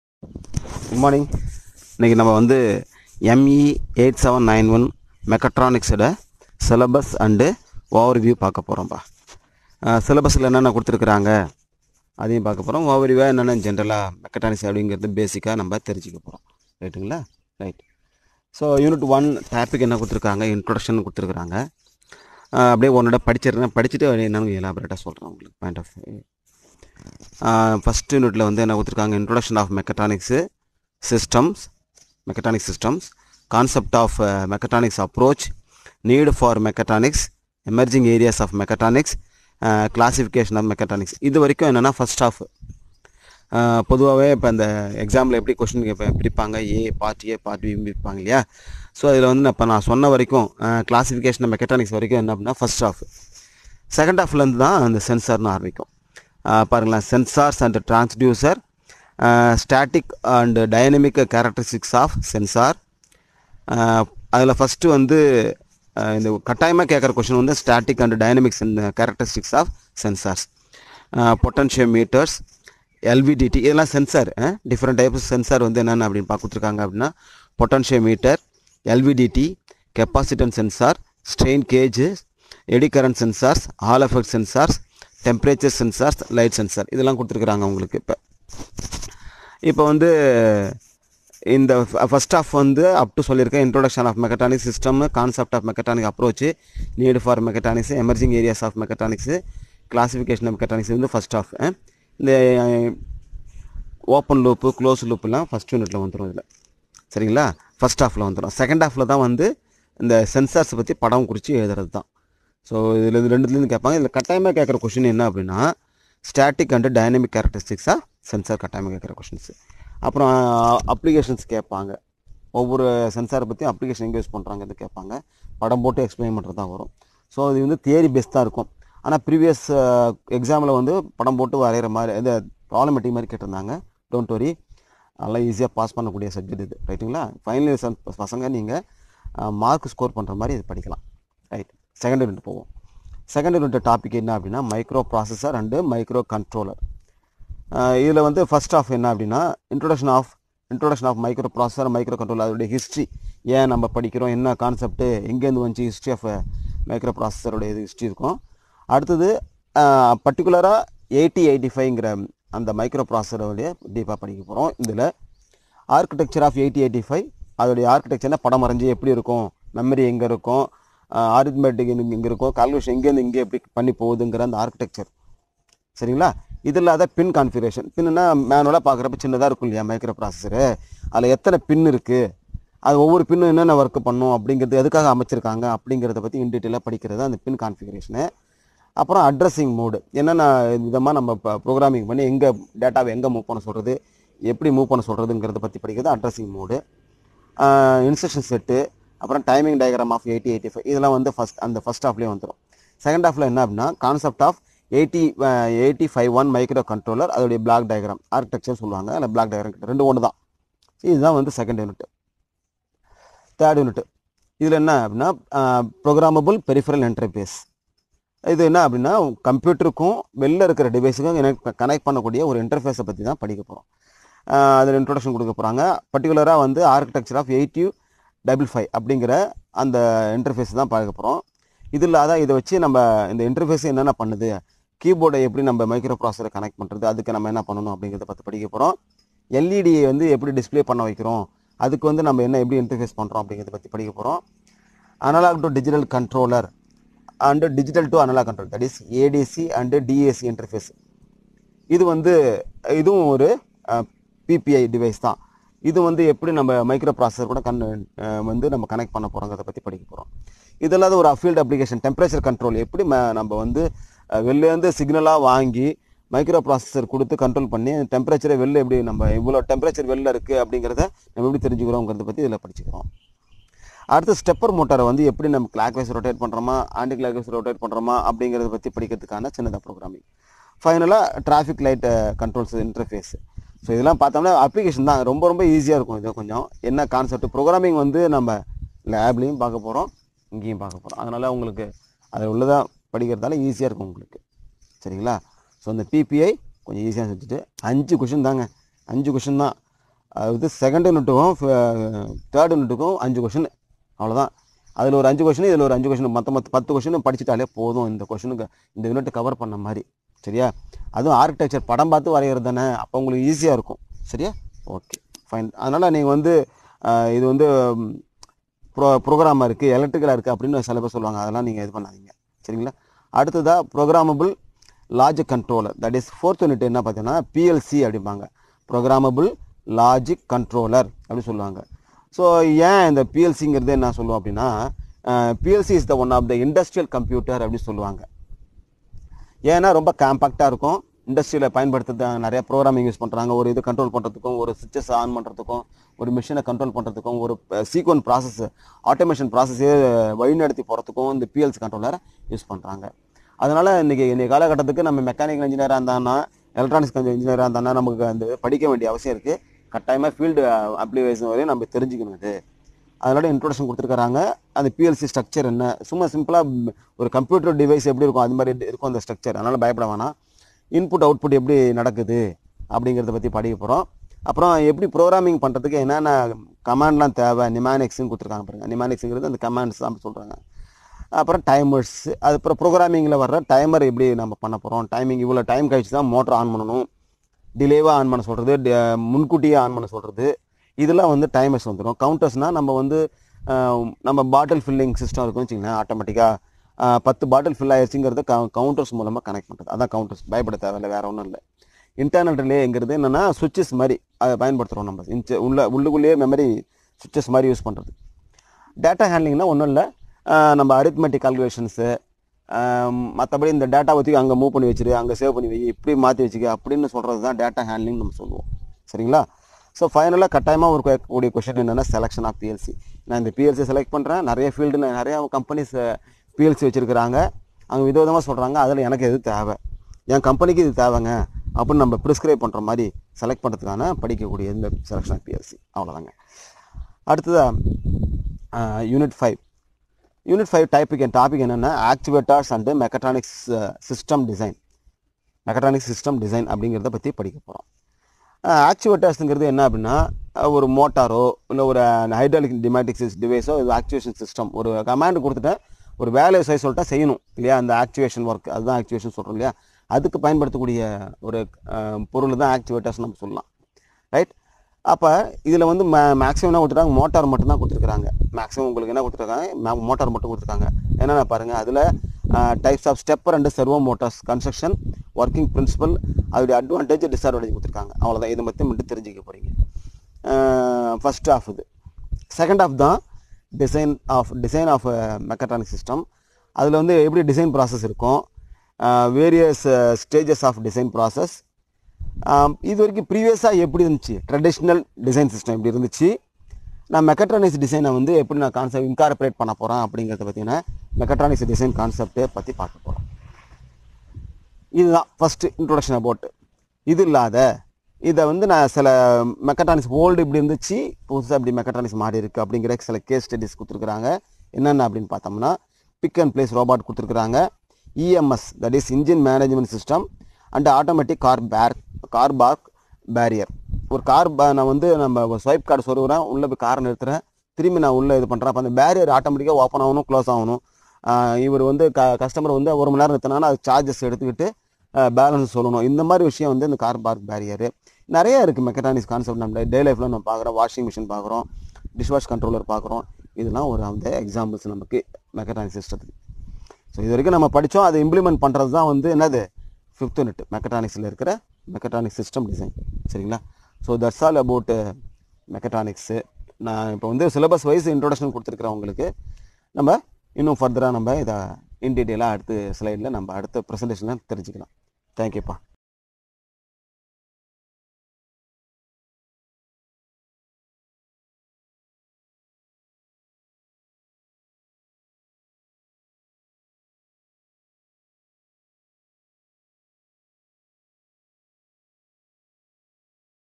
ME 8791-MECHATRONICS - SYLLABUS & OVERVIEW
Brief introduction of mechatronics subject with syllabus and overview of mechatronics subject for final mechanical engineering students